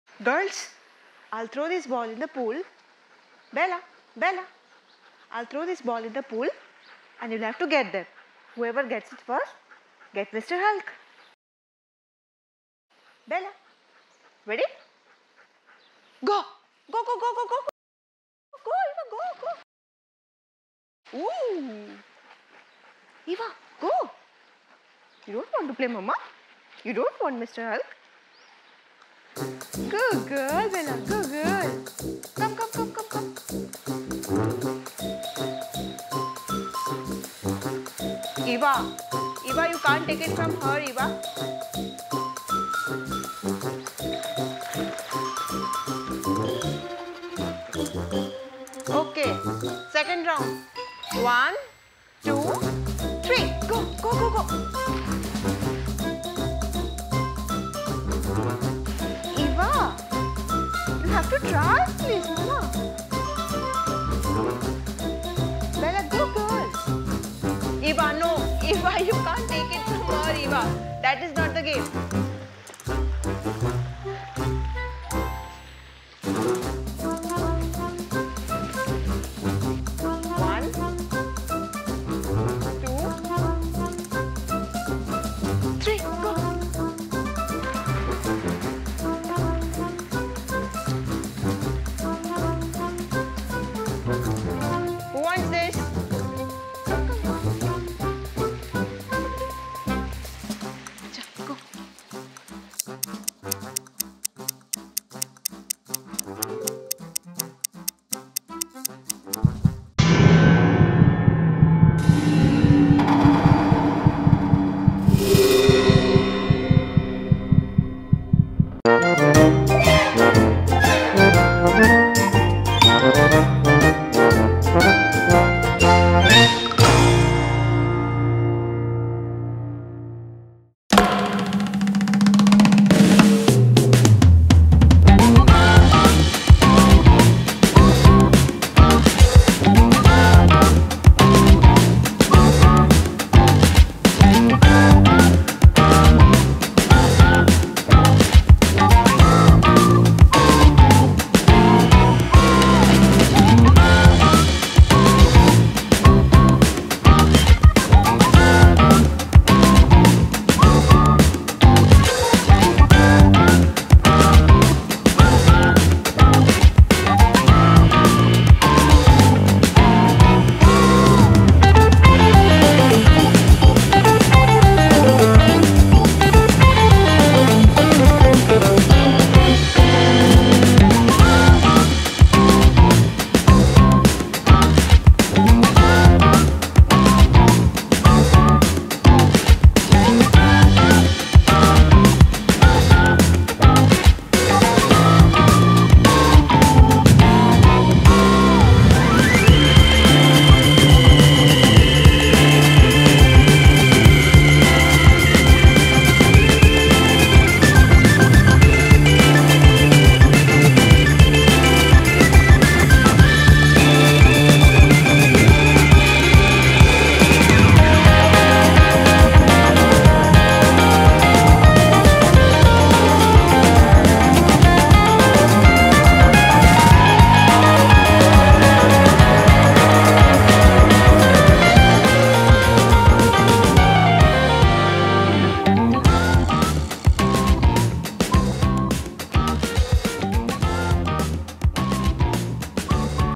My Dogs Jumping In Water To Fetch Ball Funny | Dogs Compete in Fetch Game || Eva logic
In today’s video, the challenge I gave to my dogs is fetching the ball from the pool. Whoever wins, gets a plush toy. So my dogs compete in the fetch game and they try fetching balls from the water. Later on, when the winner is declared, Eva is not willing to give up. She chases down Bella to get the plush toy.  So, my dogs, Eva and Bella, start to fight for toys (Mr. Hulk).

My Dogs Compete in Fetch Game || fetching ball from pool || Eva logic